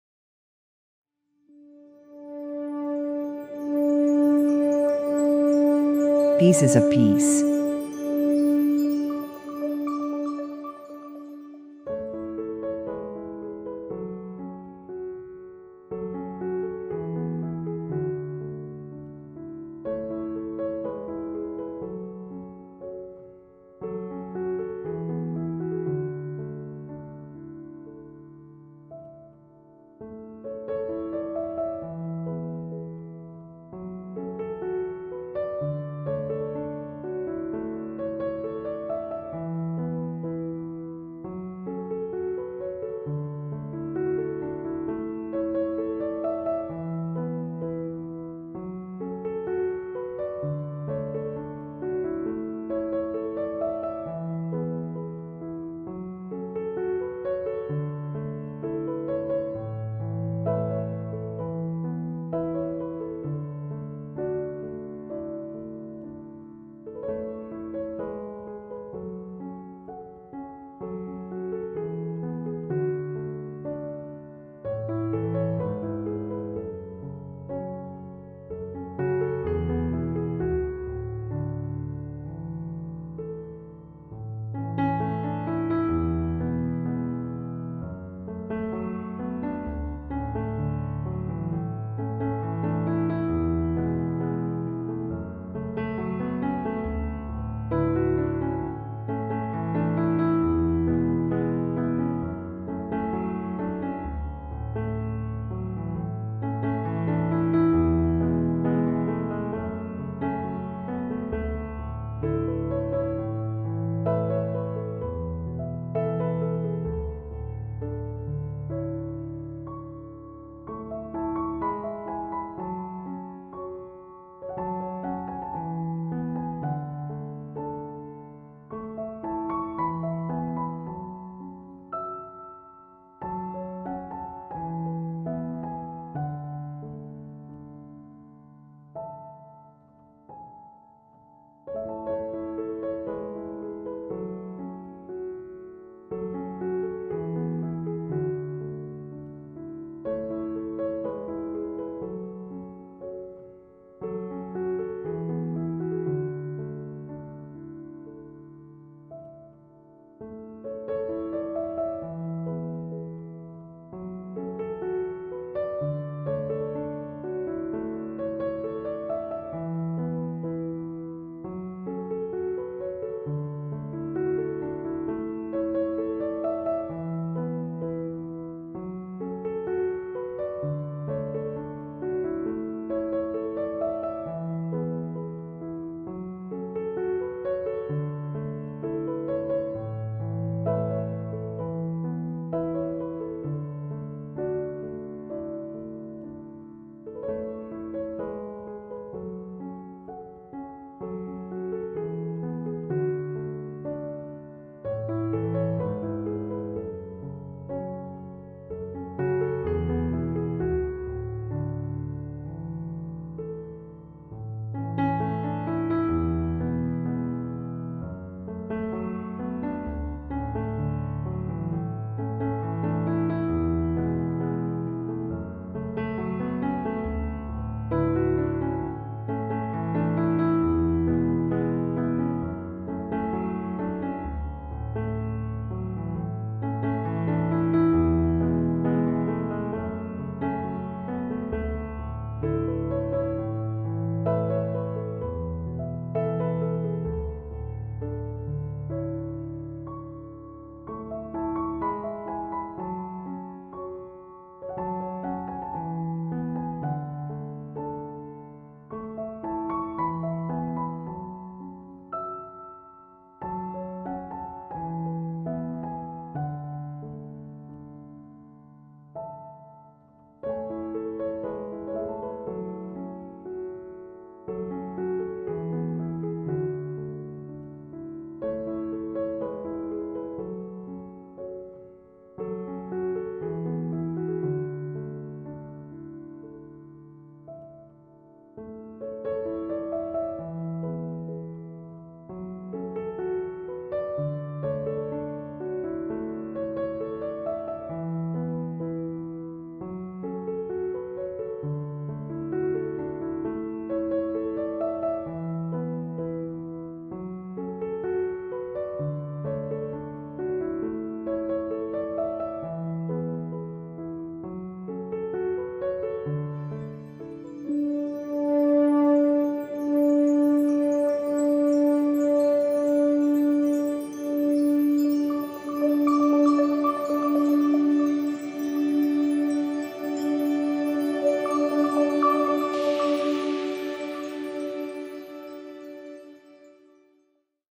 Jigsaw puzzle: Calming classical music with a mystery jigsaw puzzle (timelapse)
Calming Jazz & Blues music with a mystery jigsaw puzzle (timelapse) that can help YOU relex. Jigsaw puzzle paired with any type of calming music would be a perfect way to unwind.

🧩 In frame: ❓
🧩 Pieces: 104
🧩 Timelapse: 300%

2:18 - 50% Completion